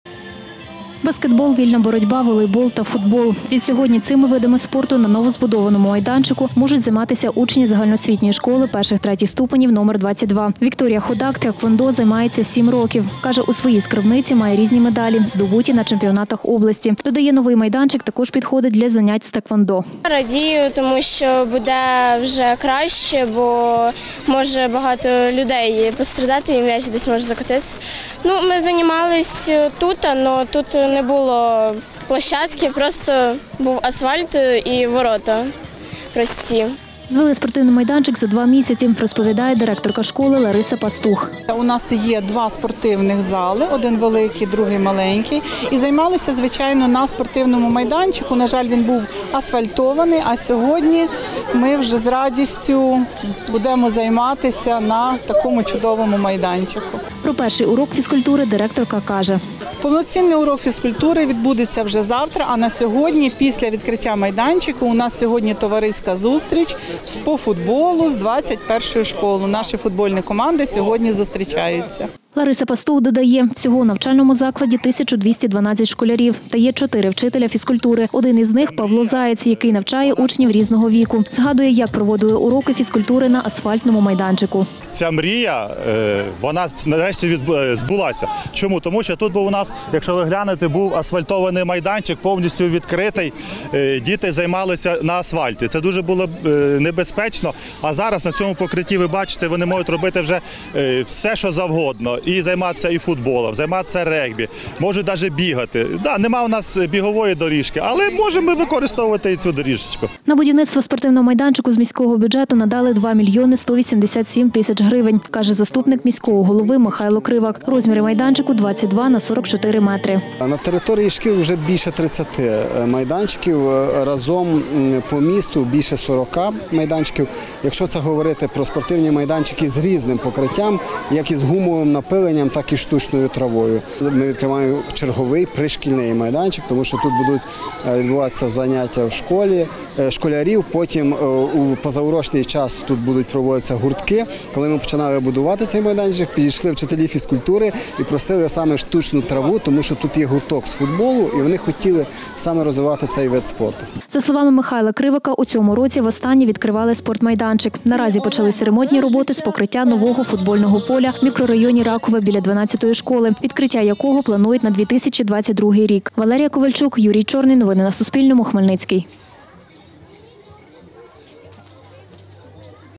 Новий спортмайданчик відкрили у Хмельницькому у школі №22
Спортивний майданчик відкрили сьогодні у Хмельницькому. Він багатофункціональний, має штучне покриття. Збудували його на території середньої загальноосвітньої школи І-ІІІ ступенів №22 імені Олега Ольжича, розповів заступник міського голови Михайло Кривак.